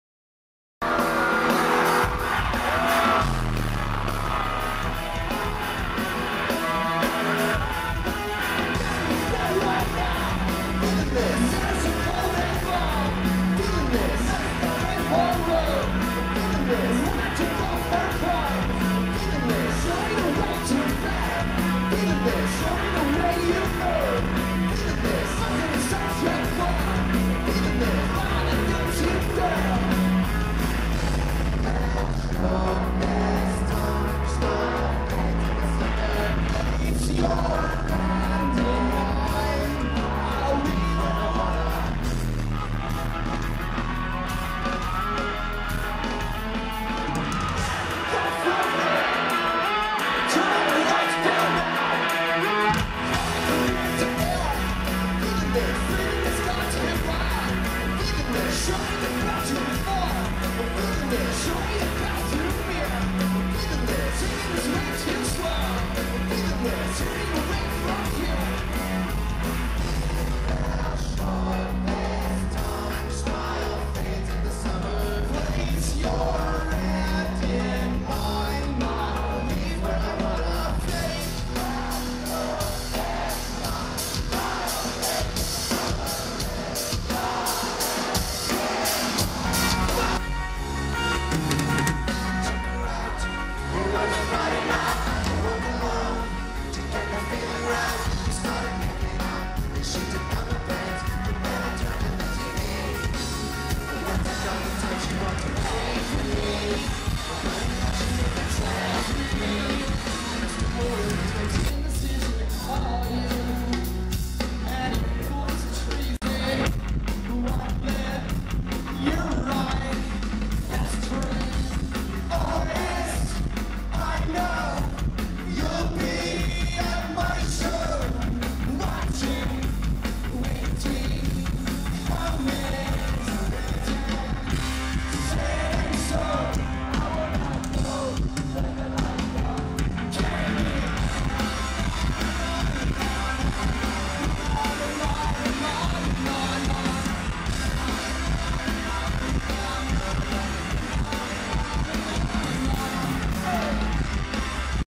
Blink 182 - Live At The LG Arena Birmingham
Blink-182 at the LG-Arena Birmingham 16/6/2012